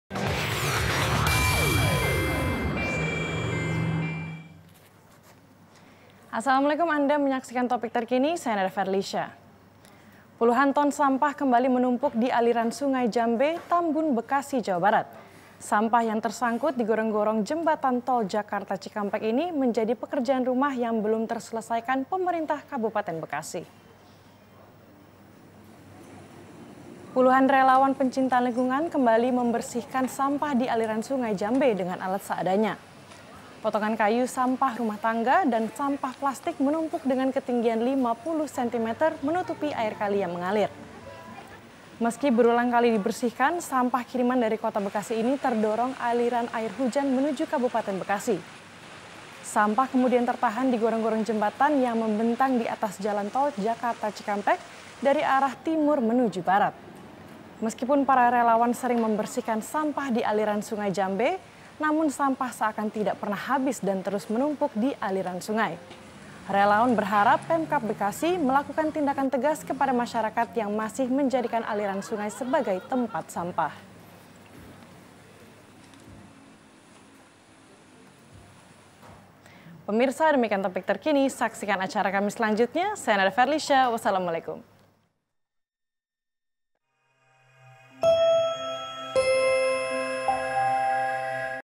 Puluhan Ton Sampah Membanjiri Kali Jambe Bekasi
Puluhan Ton Sampah Kembali Menumpuk Dialiran Sungai  Jambe Tambun Bekasi Jawa Barat. Sampah Yang Tersangkut Di Gorong-Gorong Jembatan Tol Jakrta-Cikampek Ini Menjadi Pekerjaan Rumah Yang Belum Terselesaikan  Pemerintah Kabupaten Bekasi